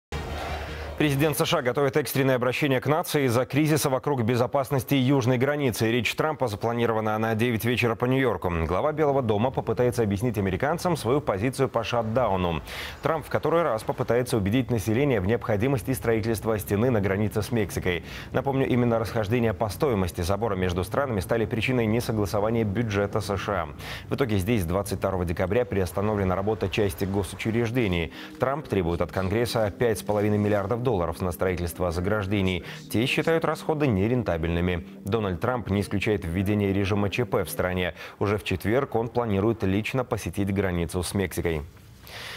Президент США готовит экстренное обращение к нации из-за кризиса вокруг безопасности южной границы
Речь Трампа запланирована на 9 вечера по Нью-Йорку. Глава Белого дома попытается объяснить американцам свою позицию по «шатдауну». Трамп в который раз попытается убедить население в необходимости строительства стены на границе с Мексикой. Именно расхождения по стоимости забора между странами стали причиной несогласования бюджета США. В итоге здесь с 22-го декабря приостановлена работа части госучреждений. Трамп требует от Конгресса 5,5 миллиардов долларов на строительство заграждений. Те считают расходы нерентабельными. Дональд Трамп не исключает введение режима ЧП в стране. Уже в четверг он планирует лично посетить границу с Мексикой.